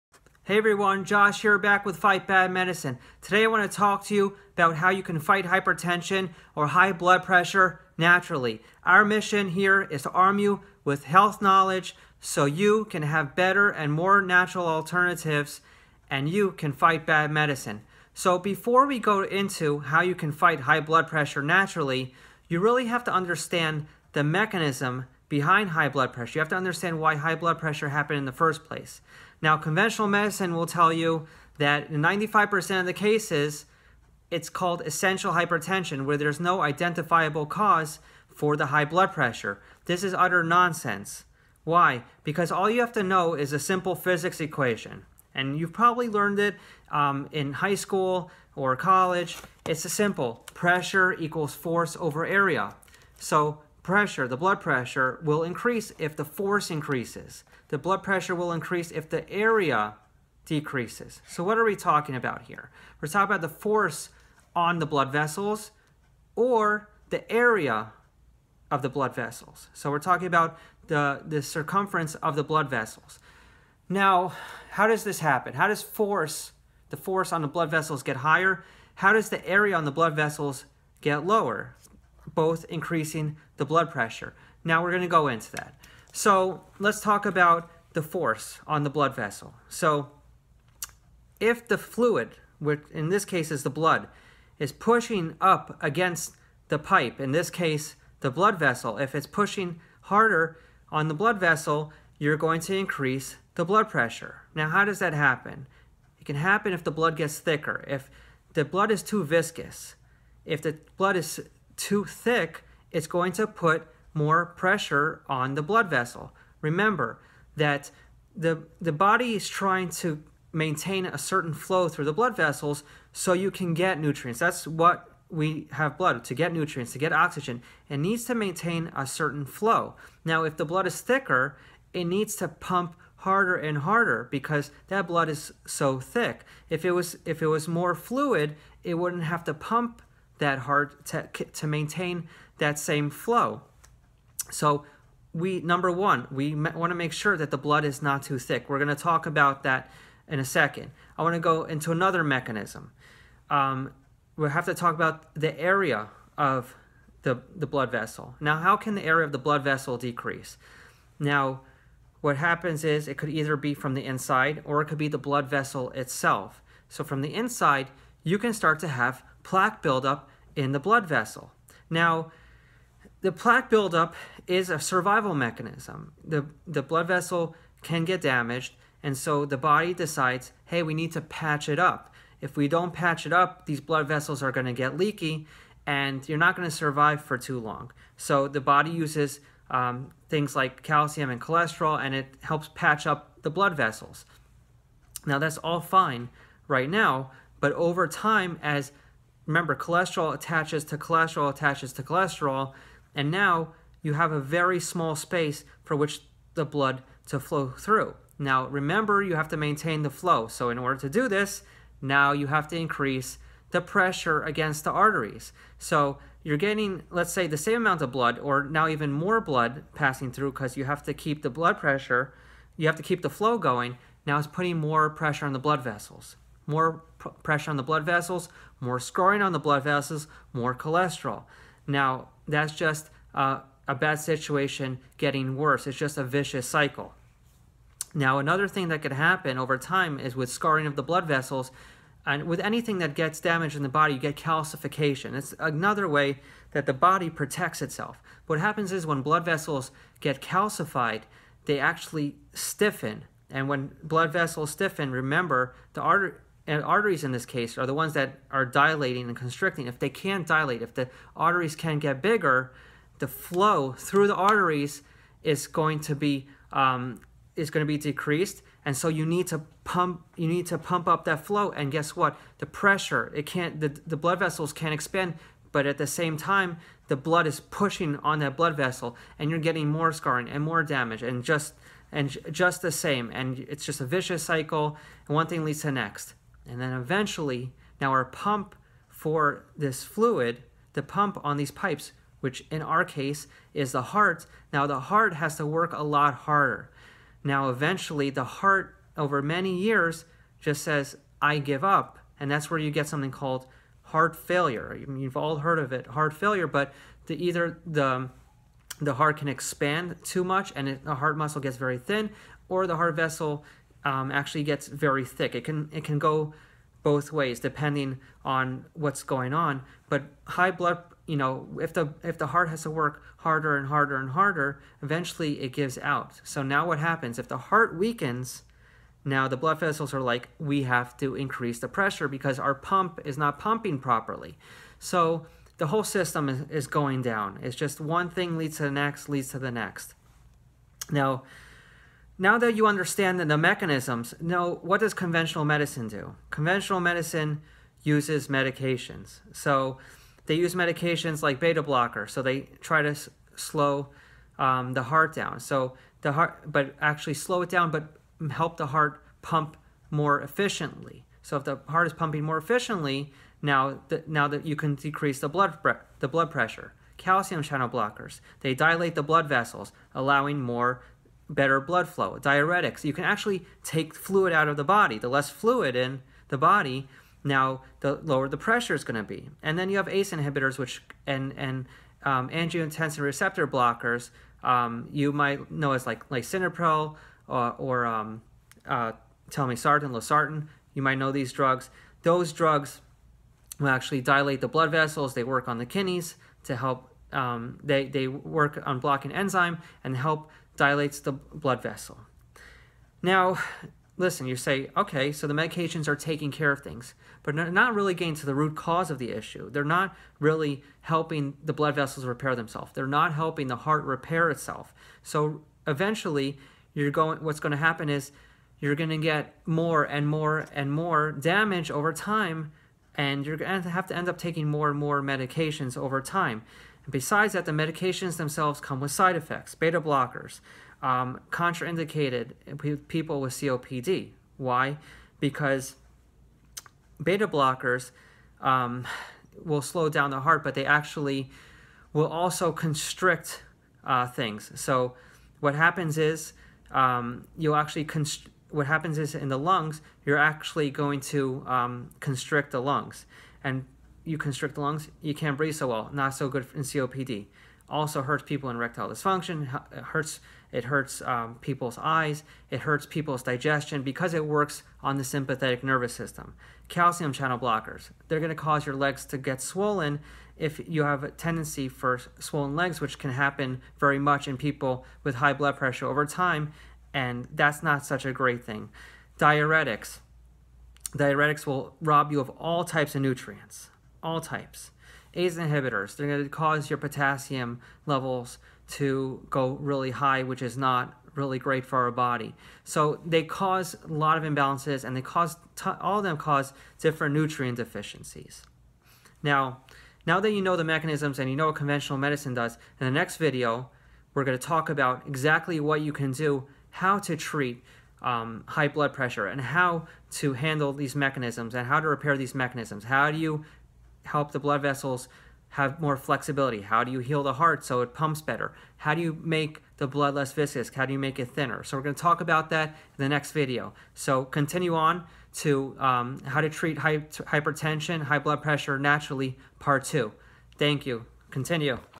How to Fight High Blood Pressure Naturally (Part 1)
In this video (Part 1) we talk about the mechanism behind blood pressure to truly understand what it is that leads to high blood pressure. In Part 2 we will talk about how to get to the root cause Naturally.